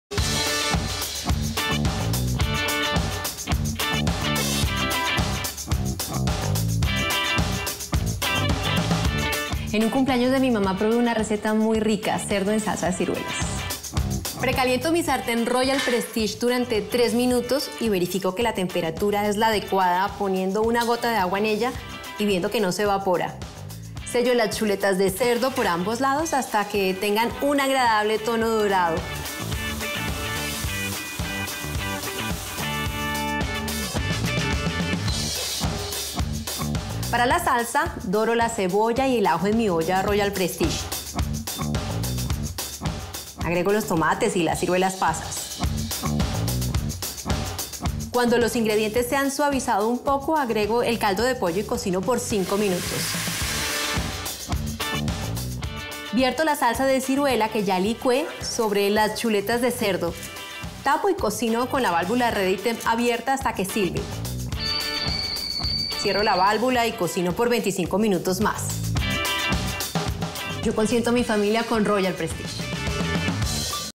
Cocina con Royal Prestige | Receta de Cerdo en salsa de ciruelas
Disfruta tus almuerzos preparando este delicioso Cerdo Agridulce en nuestros sistemas de cocina Royal Prestige. ¡Agenda una demostración en casa!

Tiempo de cocción: 40 min
Cantidad: 4 a 6 porciones
Utensilio: Sartén de  10 ½  pulgadas

Ingredientes:
1 kilo  de medallones de lomo de cerdo
3 tomates picados en cubos medianos
2 dientes de ajo finamente picados
¼  de cebolla picada en cubos
1 taza de ciruelas pasas sin semilla
2 tazas de caldo de pollo
150 gramos de tocineta finamente picada

Preparación:
1. Sellar los medallones de cerdo en la sartén Royal Prestige previamente caliente
2. Reservar y agregar sal y pimienta
3. Para la salsa, dorar la tocineta y adicionar cebolla, ajo y tomate.
4. Agregar las ciruelas pasas. Una vez que se disuelvan, incorporar el caldo de pollo y cocinar por cinco minutos. Licuar y colar.
5. Agregar la salsa a los medallones de cerdo. Tapar con la válvula Redi-Temp abierta hasta que silbe.
5. Cerrar la válvula y cocinar por 25 minutos más.

En este vídeo te enseñamos una receta fácil para que tengas más tiempo para ti y para tu familia.